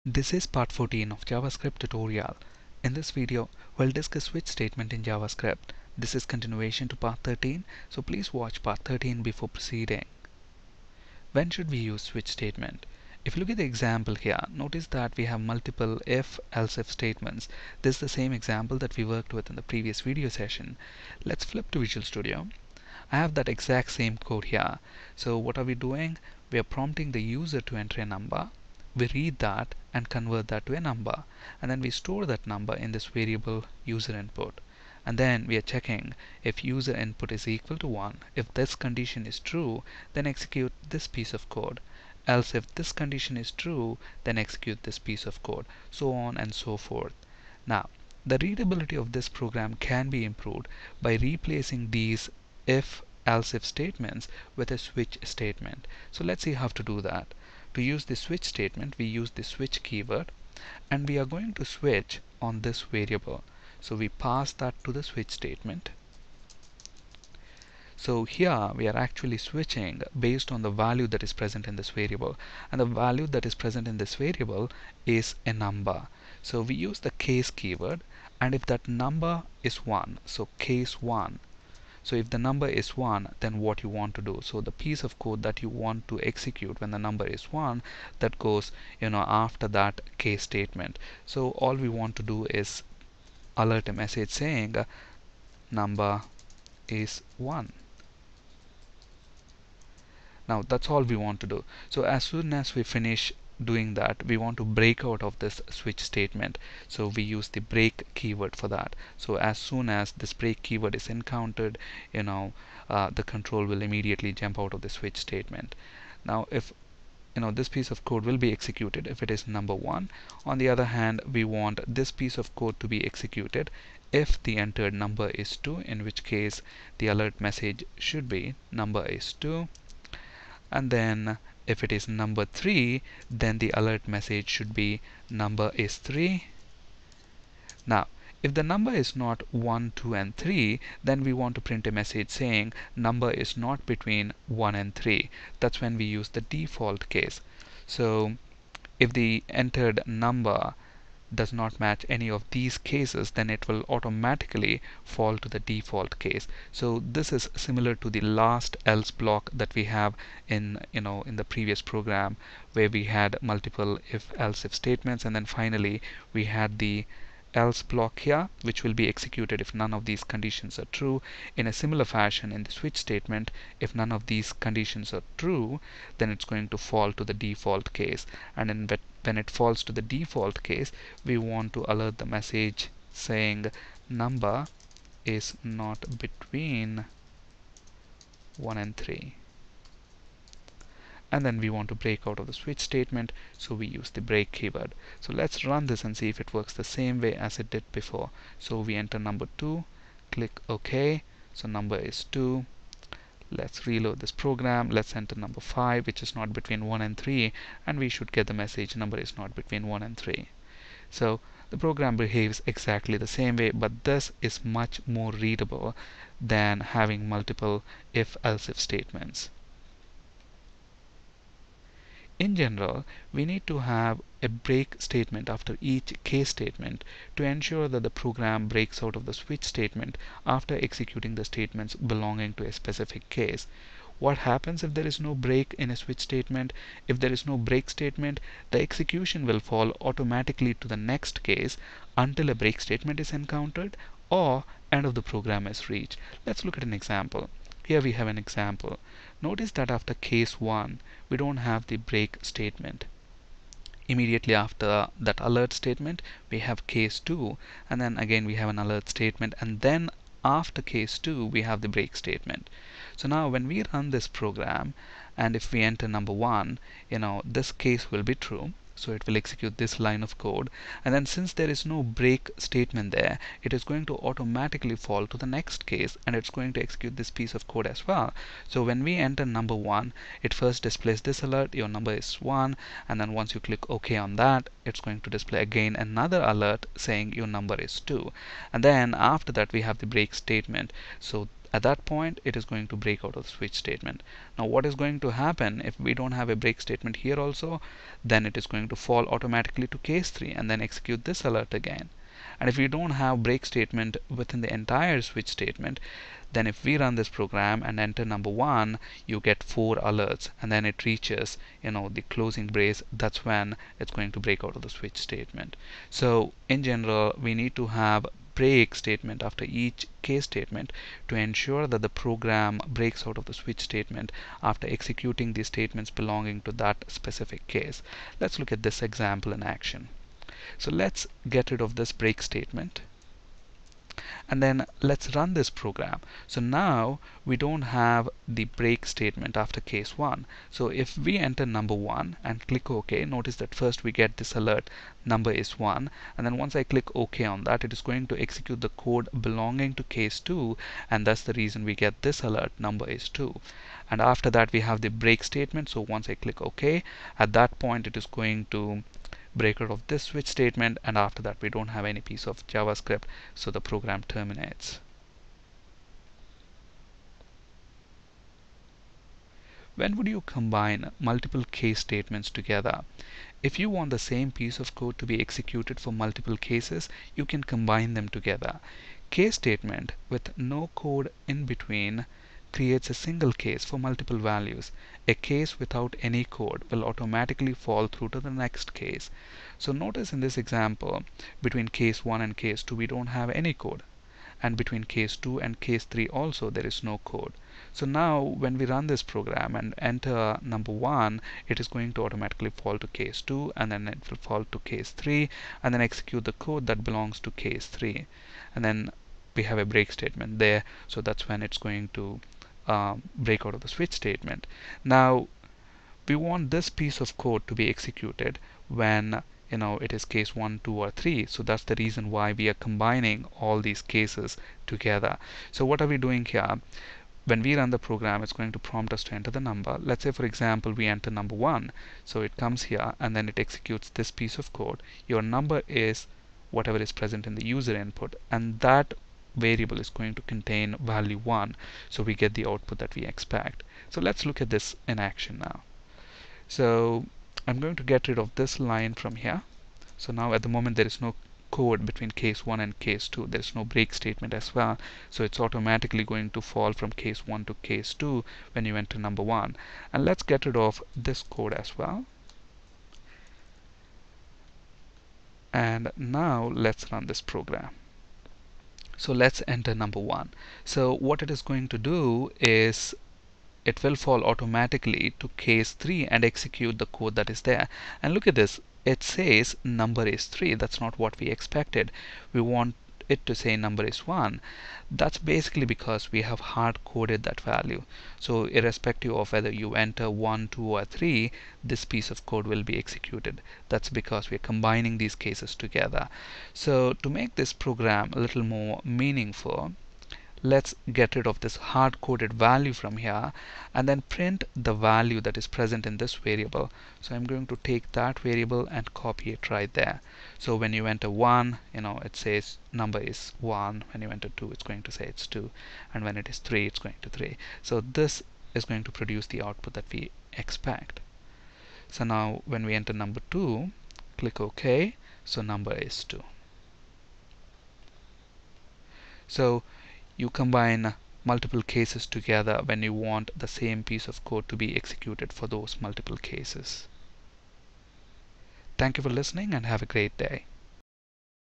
Switch statement in JavaScript
When should we use switch statement
To improve the readability of a program multiple if else if statements can be replaced with a switch statement.

Notice that in the following code we have several if else if statements
var userInput = Number(prompt("Please enter a number", ""));
if (userInput == 1)
    alert("You number is One");
else if (userInput == 2)
    alert("You number is Two");
else if (userInput == 3)
    alert("Your number is Three");
else
    alert("Your number is not between 1 and 3");

The above code can be rewritten using a switch statement and it greatly improves the readability
switch (userInput)
    case 1:
        break;
    case 2:
        break;
    case 3:
        alert("You number is Three");
        break;
    default:
        alert("You number is not between 1 and 3");
        break;

In general we need to have break statement after each case statement to ensure the program breaks out of the switch statement after executing statements present in a specific case.

What happens if there is no break in a switch statement?
If there is no break statement the execution falls automatically to next case until a break statement is encountered or end of the program is reached.

In the example below, since we don't have a break statement for case 1, when we enter 1 as the number we would get 2 alerts. First alert from case 1 and the second alert from case 2.
switch (userInput)
    case 1:
    case 2:
        break;
    case 3:
        break;
    default:

When would you combine multiple case statements together?
If you want the same piece of code to be executed for multiple cases you can combine them together as shown below. Case statement with no code in-between creates a single case for multiple values. A case without any code will automatically fall through to the next case.

In this example case 1 and case 2 will fall through and execute code for case 3
switch (userInput)
    case 1:
    case 2:
    case 3:
        alert("You number is "+ userInput);
        break;
    default: